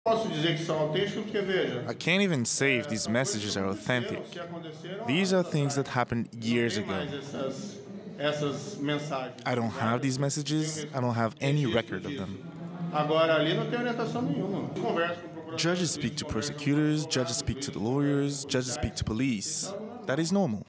Brazil justice minister Sergio Moro denies conspiring against Lula
"I can't even say that these messages are authentic", reacts Sergio Moro, Brazilian. He denies claims he conspired to keep former president Luiz Inacio Lula da Silva out of the 2018 election.